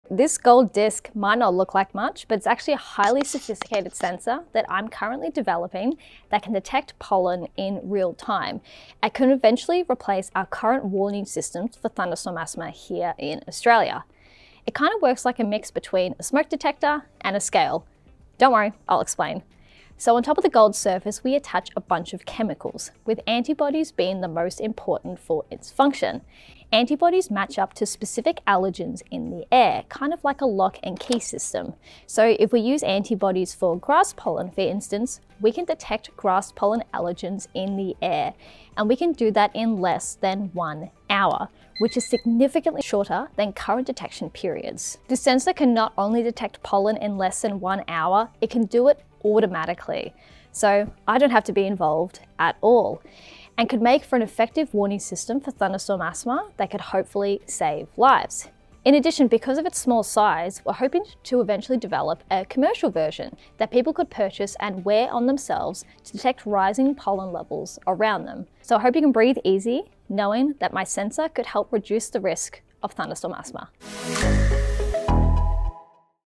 Breathe easy: Melbourne’s thunderstorm asthma breakthrough
As Spring approaches, so does the season for hay fever. For allergy sufferers, there's exciting news: a Deakin PhD student is developing an innovative sensor that could offer real-time monitoring of air quality and pollen levels.